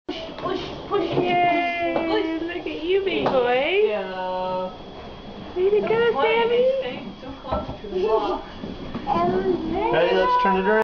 09 months - Learning to Walk
Sam started walking with his walker toys to try to keep up with Garrett